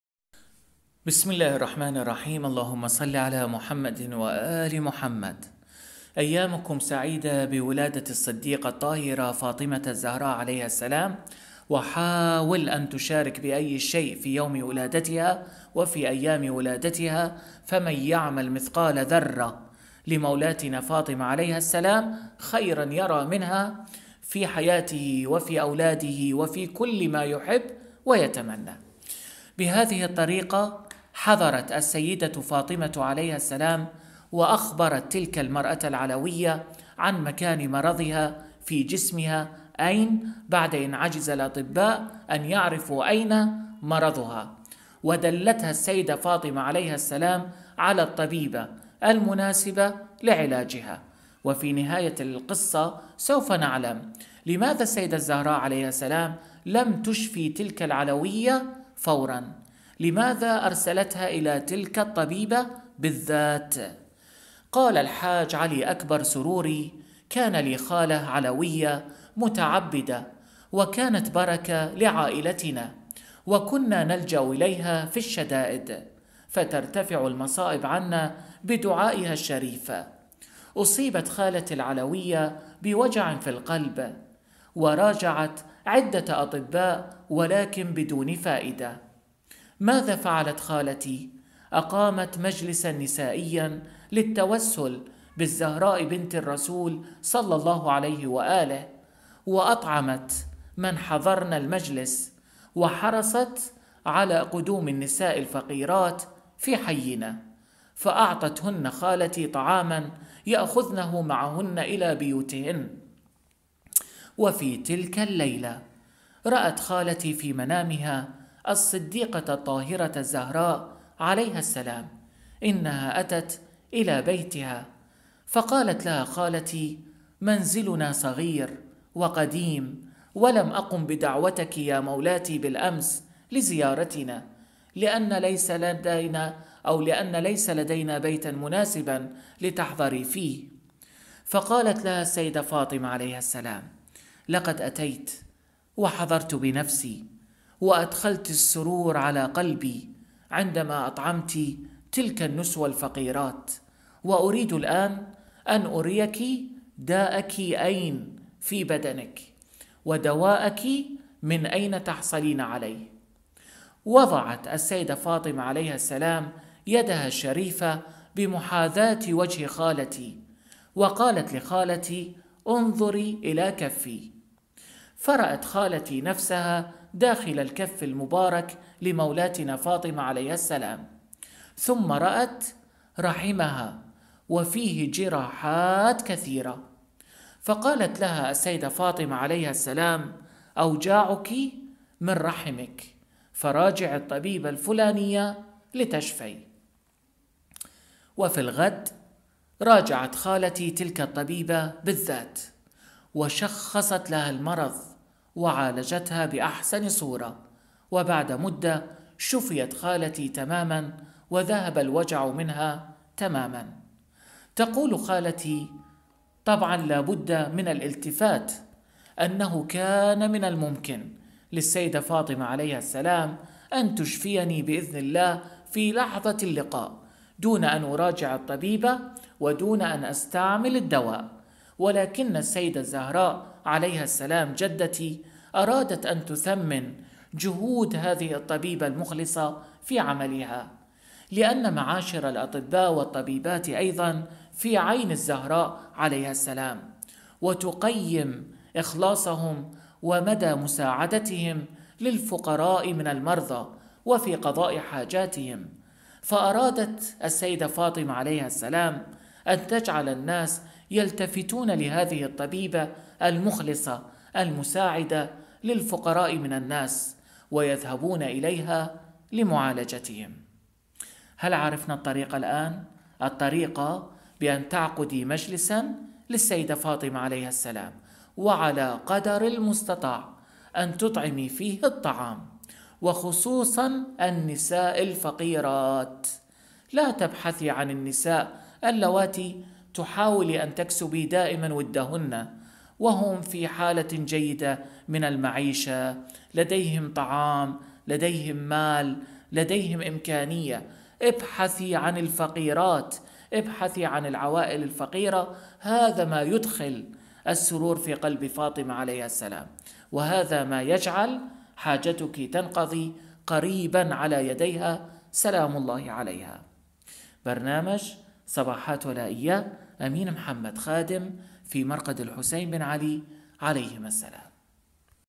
أكثر الطرق نفعا كي تقضي مولاتنا فاطمة عليها السلام حاجتك وتعطيك حتى إسم الشخص الذي تراجعه لقضاءها
أهل البيت عليهم السلام هم الحل لكل مشكلة وأيامكم سعيدة بولادة السيدة فاطمة الزهراء عليها السلام ونرجو الشفاعة منها ومن أولادها الأئمة المعصومين عليهم السلام في الدنيا والآخرة.